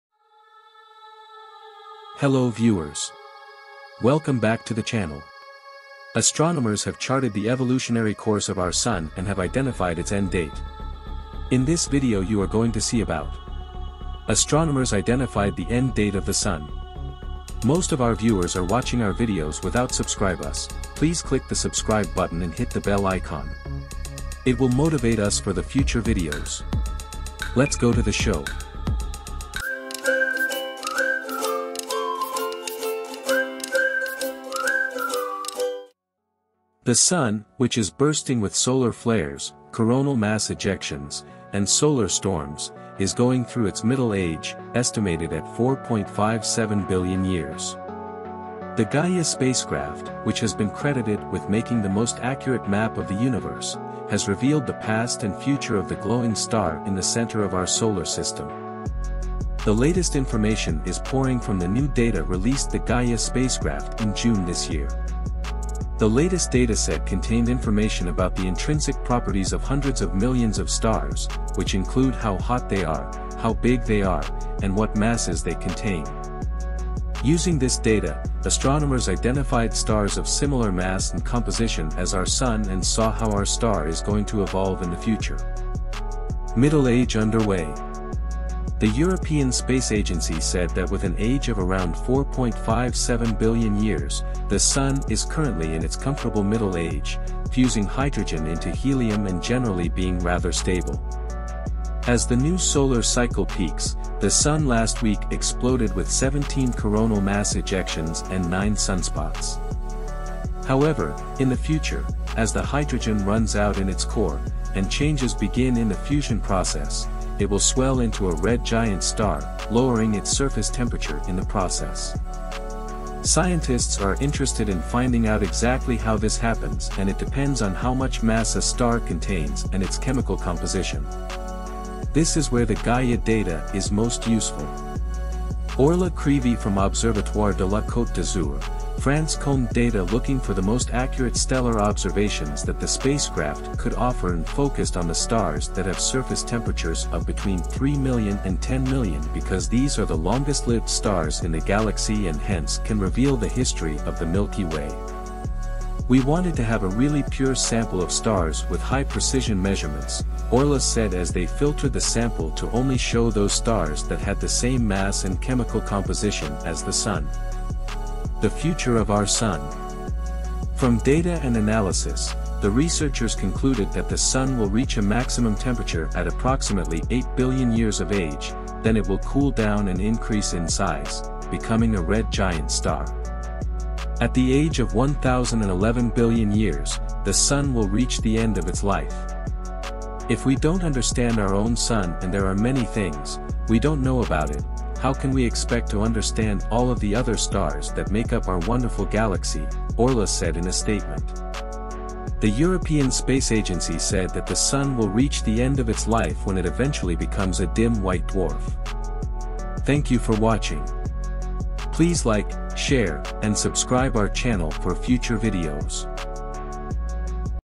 End of The Sun - Astronomers Identified the end date of the Sun
The Sun, which is bursting with solar flares, coronal mass ejections, and solar storms, is going through its middle age, estimated at 4.57 billion years. The Gaia spacecraft, which has been credited with making the most accurate map of the universe, has revealed the past and future of the glowing star in the center of our Solar System.
The latest information is pouring from the new data released the Gaia spacecraft in June this year. The latest dataset contained information about the intrinsic properties of hundreds of millions of stars, which include how hot they are, how big they are, and what masses they contain. Using this data, astronomers identified stars of similar mass and composition as our Sun and saw how our star is going to evolve in the future. Middle-age underway.
The European Space Agency said that with an age of around 4.57 billion years, the Sun is currently in its comfortable middle age, fusing hydrogen into helium and generally being rather stable. As the new solar cycle peaks, the Sun last week exploded with 17 coronal mass ejections and nine sunspots. However, in the future, as the hydrogen runs out in its core, and changes begin in the fusion process, it will swell into a red giant star, lowering its surface temperature in the process.
Scientists are interested in finding out exactly how this happens and it depends on how much mass a star contains and its chemical composition. This is where the Gaia data is most useful.
Orlagh Creevey from Observatoire de la Côte d’Azur, France combed data looking for the most accurate stellar observations that the spacecraft could offer and focused on the stars that have surface temperatures of between 3Mn and 10Mn because these are the longest-lived stars in the Galaxy and hence can reveal the history of the Milky Way.
“We wanted to have a really pure sample of stars with high precision measurements,” Orlagh said as they filtered the sample to only show those stars that had the same mass and chemical composition as the Sun.
The future of our Sun From data and analysis, the researchers concluded that the Sun will reach a maximum temperature at approximately 8 billion years of age, then it will cool down and increase in size, becoming a red giant star. At the age of 1011 billion years, the Sun will reach the end of its life.
"If we don't understand our own Sun and there are many things, we don't know about it, how can we expect to understand all of the other stars that make up our wonderful galaxy,” Orlagh said in a statement.
The European Space Agency said that the Sun will reach the end of its life when it eventually becomes a dim white dwarf.